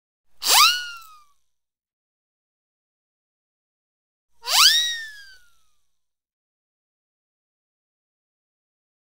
Short Bursts from Twirling High Pitched Whoopee Whistle
Short Bursts from Twirling High Pitched Whoopee Whistle · The Hollywood Edge Sound Effects Library

Animation Sound Effects, Vol. 1